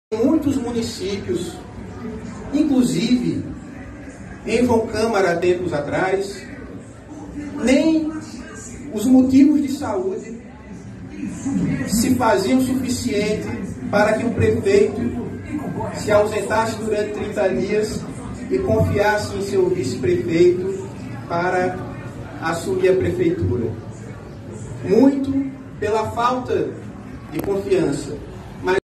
Prefeito em exercício de João Câmara "Nem os problemas de saúde afastava prefeito do cargo em JC
Atual Prefeito em exercício de João Câmara diz "Nem os problemas de saúde conseguiram afastar os prefeitos dos cargo, por falta de confiança